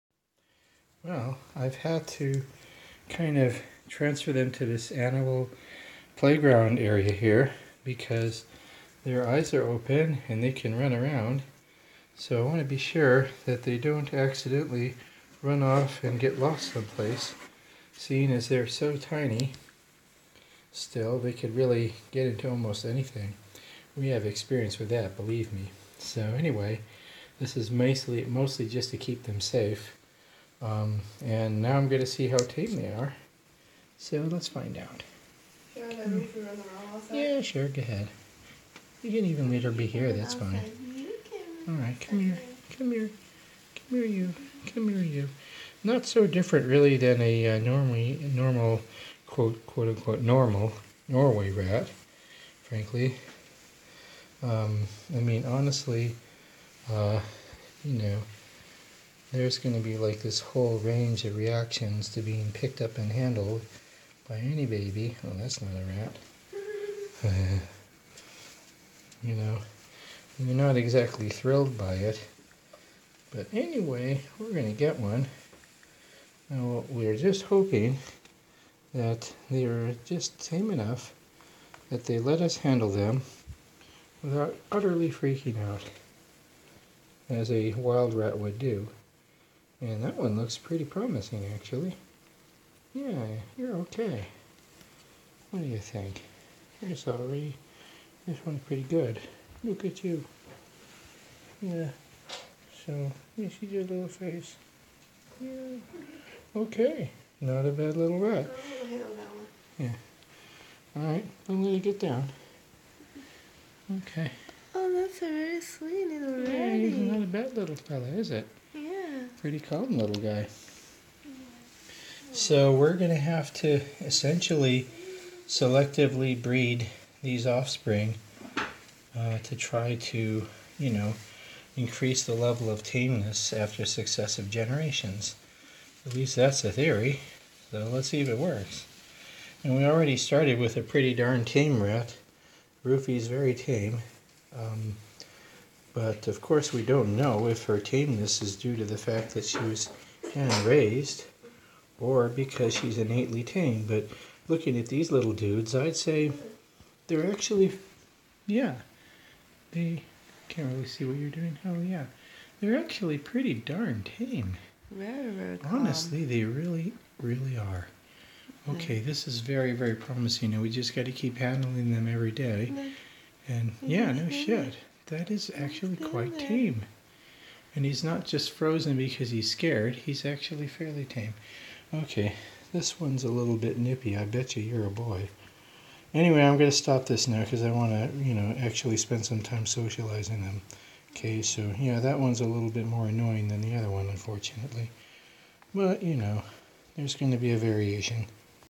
Socializing Roofy's litter of baby Roof Rats
Roofy's babies are still letting me scoop them up, handle and even cuddle with them.  Some of them are a bit squirmy while others are very happy to stand still and be petted.  Which is not unlike a typical litter of Norway rats.

I would say that some of them seem maybe just a bit more independant and "pushy" than a Norway rat would be at this age.  By pushy, I mean literally pushing with their muzzle the squeeze in someplace they want to be (frequently my sleeve), or push my finger away if I'm bugging them.  This is also something Norway rat babies will do, but they seem more adapt at it for their age.

If some aspects of their development are advanced, it is not surprising, as wild species experience selective pressure to develop certain skills faster than a domesticated species leading a more sheltered life.  A longer childhood (and more playfulness) is often associated with tameness in animals.

If this is also true in Rattus Rattus, then selective breeding for tameness could possibly involve selecting those individuals which were slower to mature than others.  But I have to take care that I wouldn't be accidently selecting the less healthy or intelligent individuals.